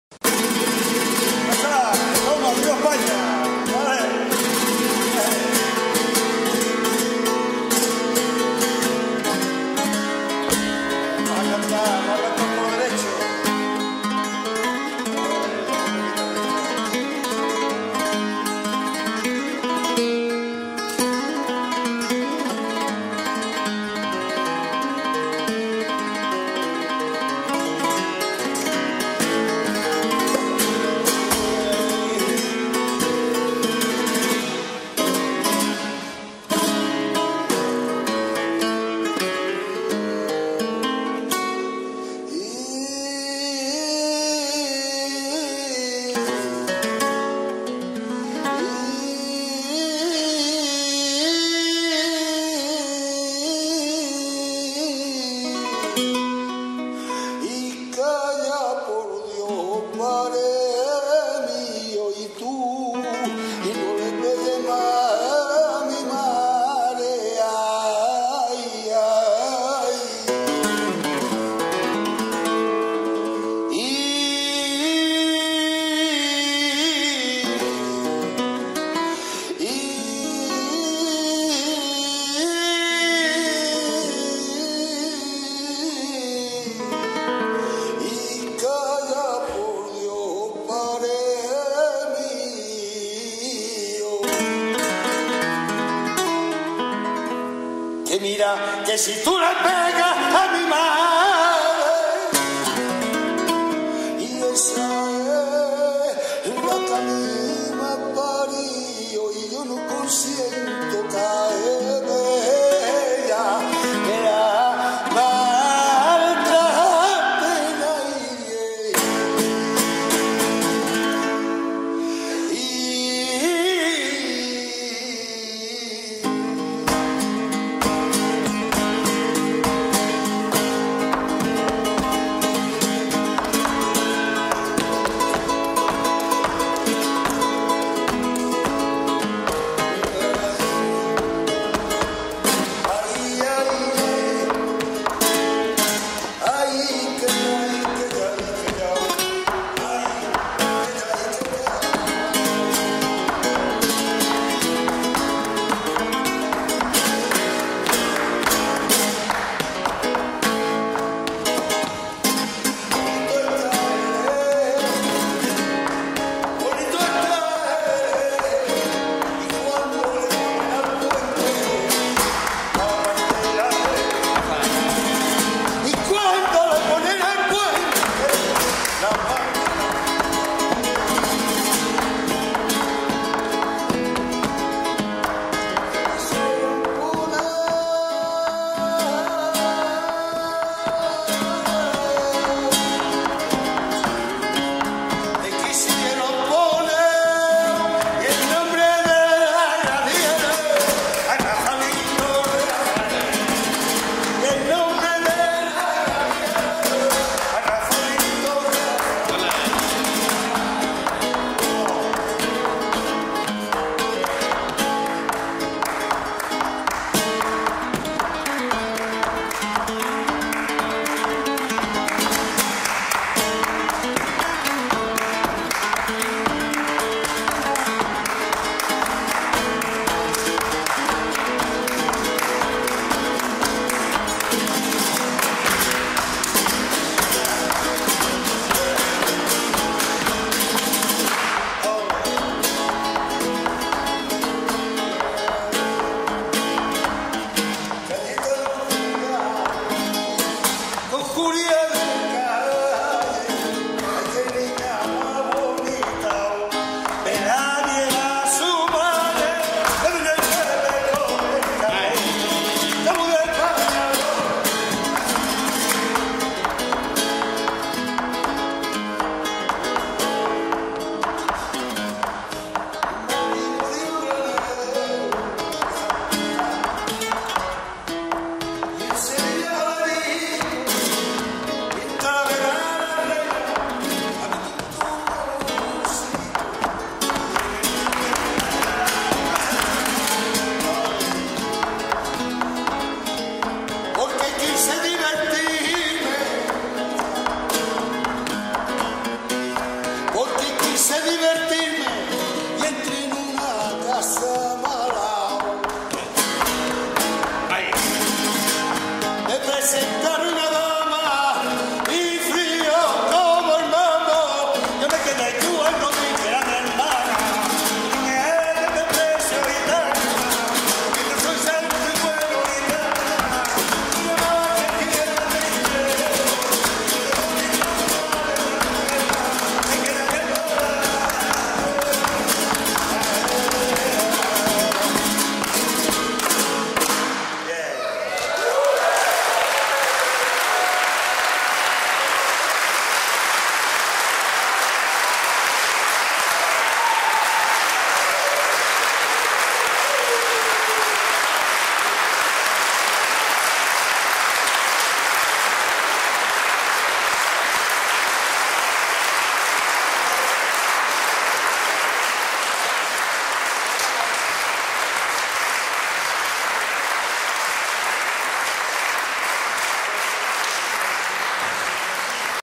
Rafael Segura y Juan Ramón Caro - 2. Teil Fandango nat. -- Bulerias
Die Symbiose oder das Zusammenschmelzen von zahlreichen Kulturen die im Laufe der Jahrhunderte nach Spanien gekommen sind, ist Flamenco.
 Andalucía Flamenca ist der Inhalt des Programms. Es handelt sich um die Darstellung des Flamencos in seiner reinsten Form.